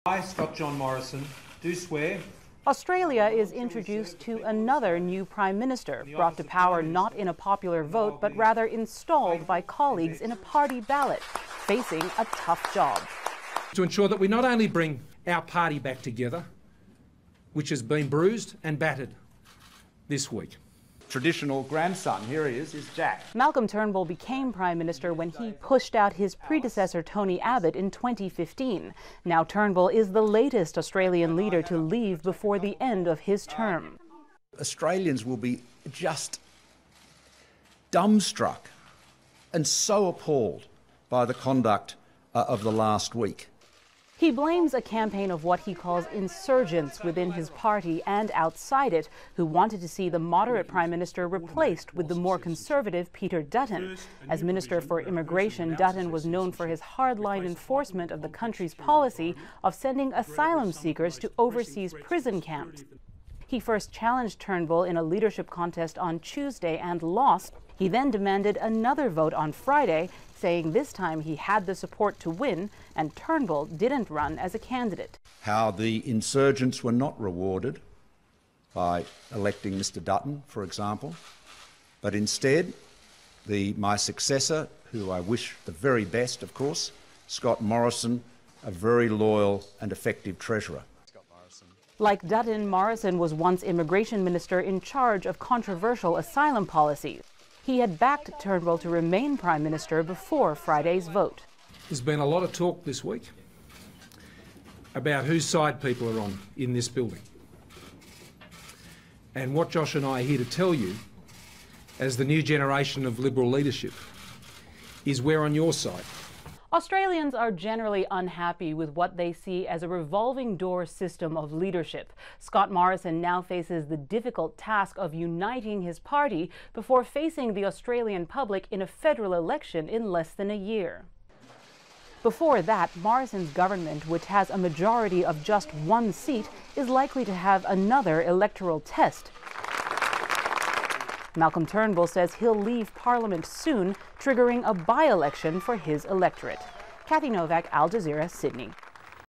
🇦🇺 Scott Morrison in as new prime minister of Australia | AL Jazeera English
Australia has a new prime minister, after the ruling Liberal Party replaced its leader.
Scott Morrison defeated Peter Dutton in an internal party vote.
It’s the fourth time since 2010 that a prime minister has been voted out of the job by his or her own party.
Al Jazeera’s Kathy Novak reports from Sydney.